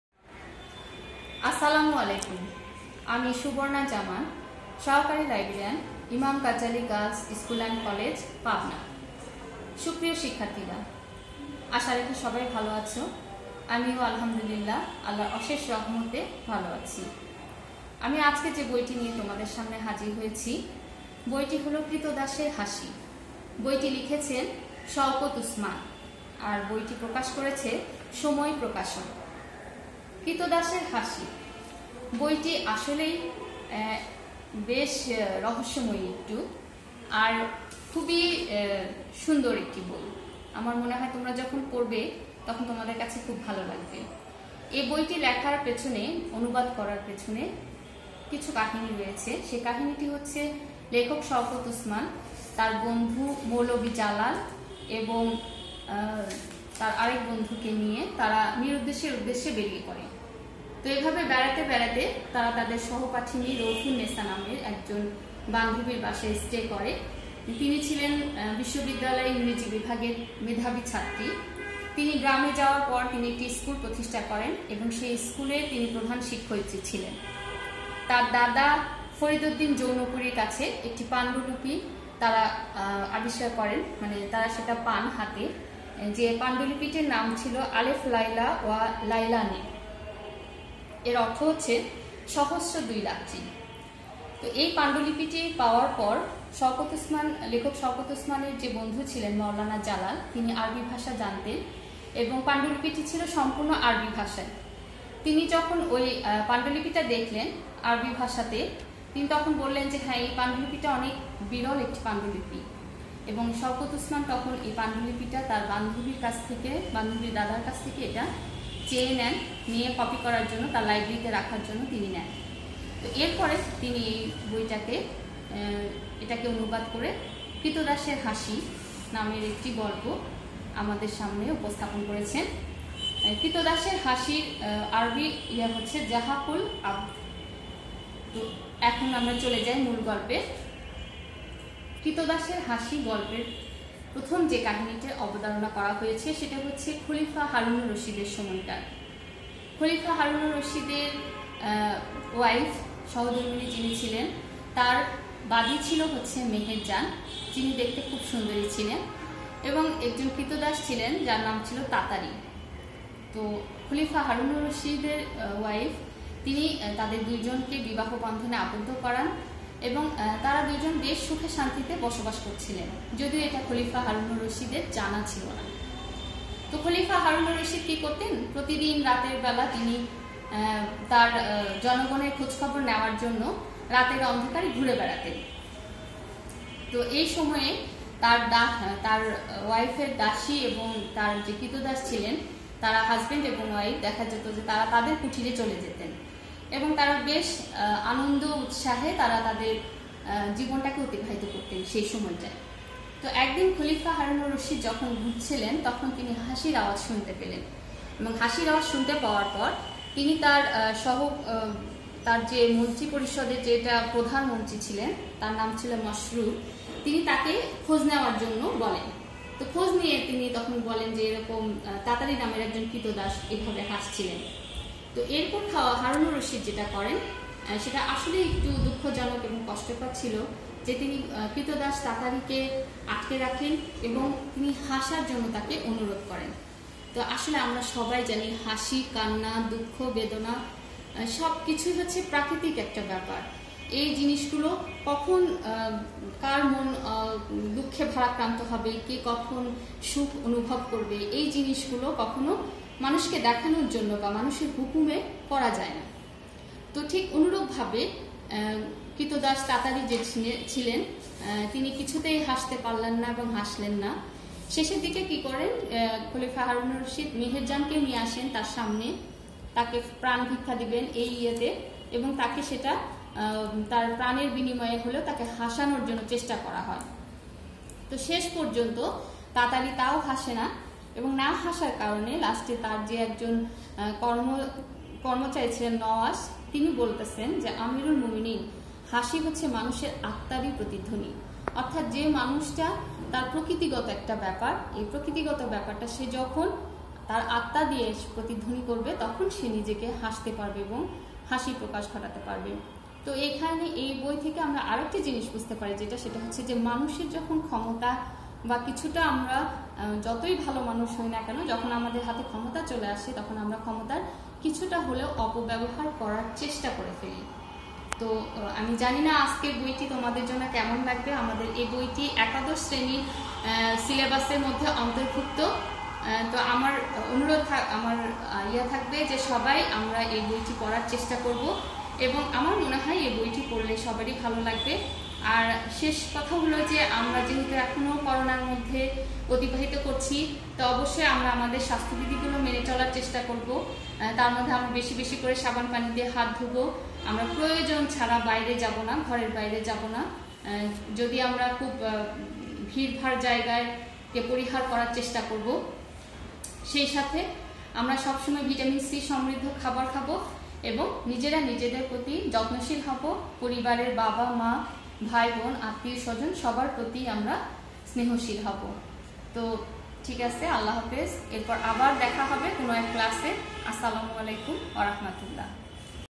লাইব্রেরি ক্লাস-০৪ _ বইঃ ক্রীতদাসের হাসি। আনুবাদকঃ শওকত ওসমান। Lecture ft. Subarna Jaman
ইমাম গাযযালী গার্লস স্কুল এন্ড কলেজ অনলাইন ক্লাসের লাইব্রেরি ক্লাসের ৪র্থ ক্লাস। ক্লাস নিচ্ছেন উক্ত কলেজের সহকারী লাইব্রেরিয়ান সুবর্না জামান।